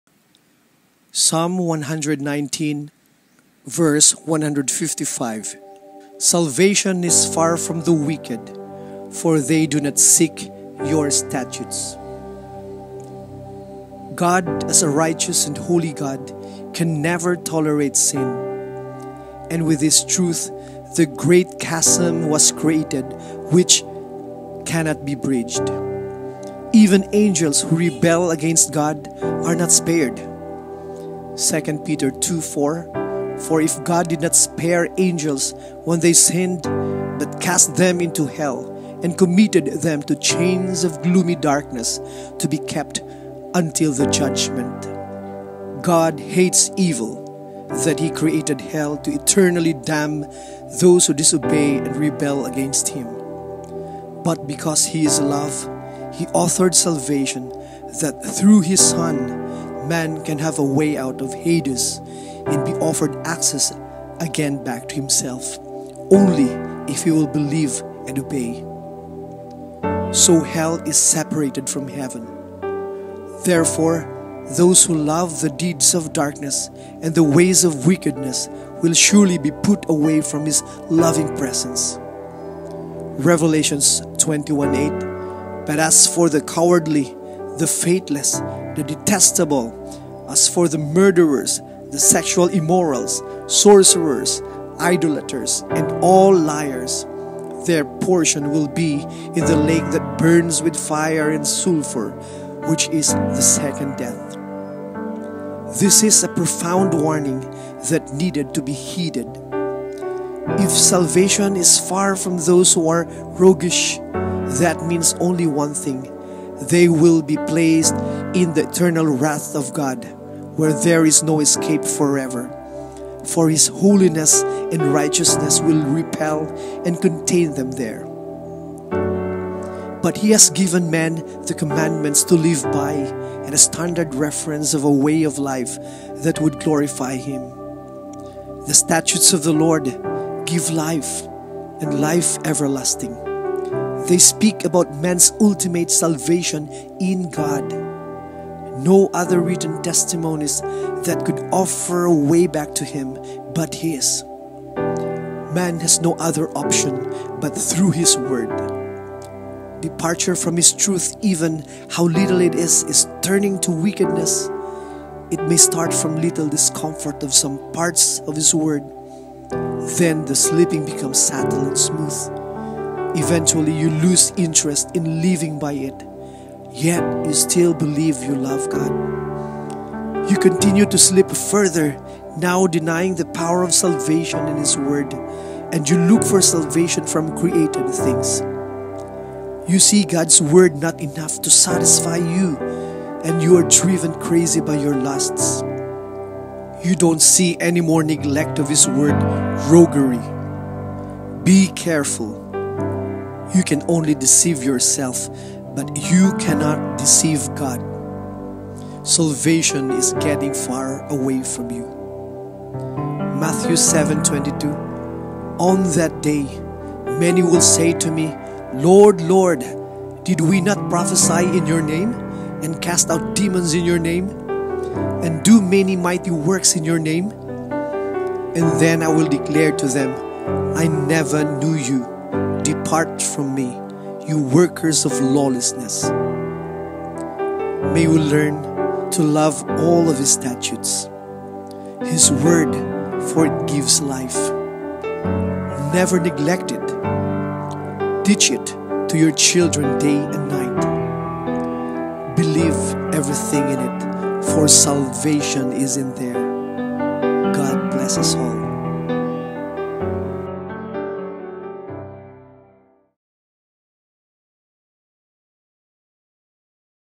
This is a warning to be heeded! While there is time trust HIS WORD for salvation can be found in it.
Psalm 119:155  Salvation is far from the wicked, for they do not seek your statutes. JESUS saves! Now is the day of salvation!